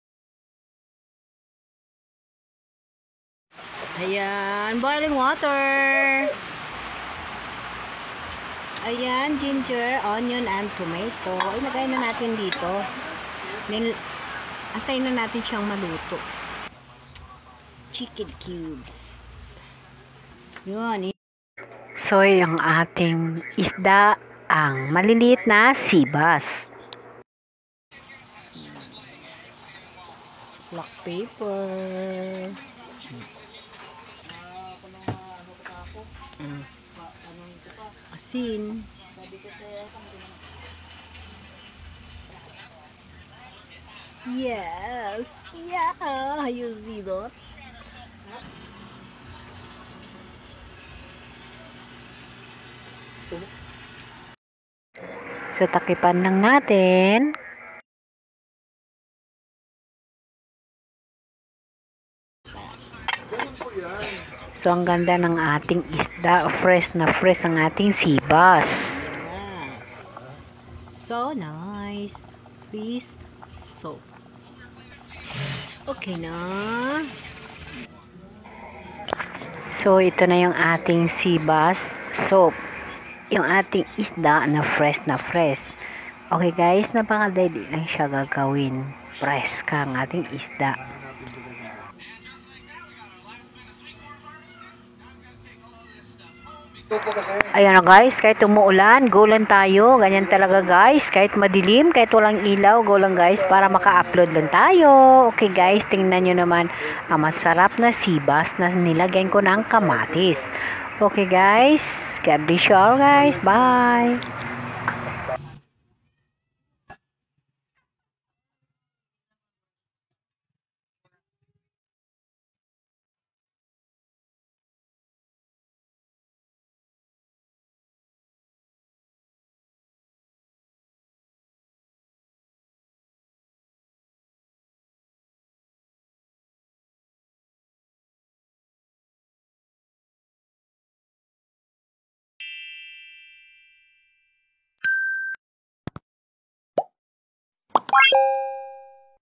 SEABASS SOUP( very simple fish soup that is so healthy Foods that help a lot with haftbody Numbness
These are the foods I eat to recover immediately from the numbness of my half body

Seabass
tomato
onion
chicken cube
black pepper
boiling water
iodizesalt salt